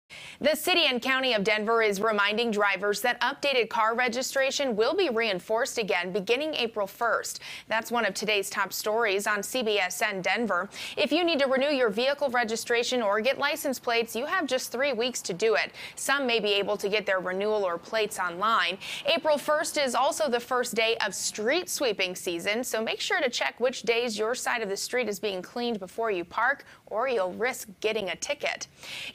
Denver Resumes Vehicle Registration Enforcement On April 1
The City and County of Denver is reminding drivers that updated car registration will be reinforced again beginning April 1.